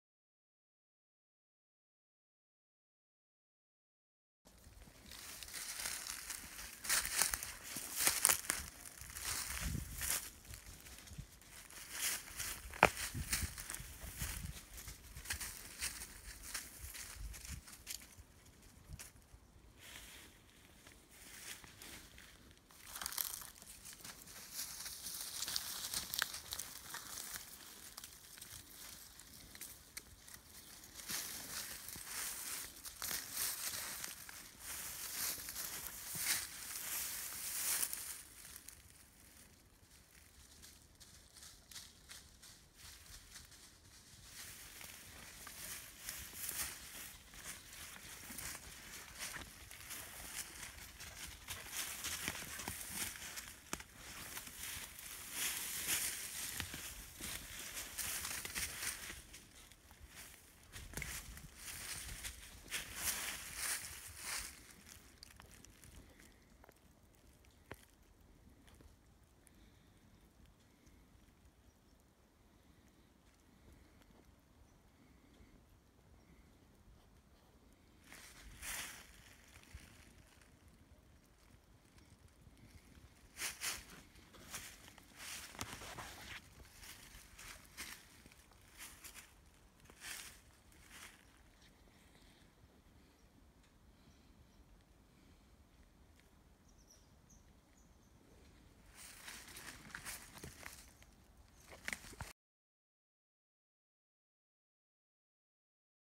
What Animal Shredded This Tree!? Northern, Maine
The last week of October, 2021 - I saw this SHREDDED tree off the side of a trail deep in the woods at Camp Oot-Oot  in Maine.  Whatever did it was able to reach (or climb) up 8 feet...  I'm guessing a moose, as the area is thick with them and I have some big bulls on the trail cams.  But there's also big whitetail bucks, bear, fisher cats, porcupines, etc.  Someone even suggested maybe it was lightening, but I don't see any burning.  Any idea what did it?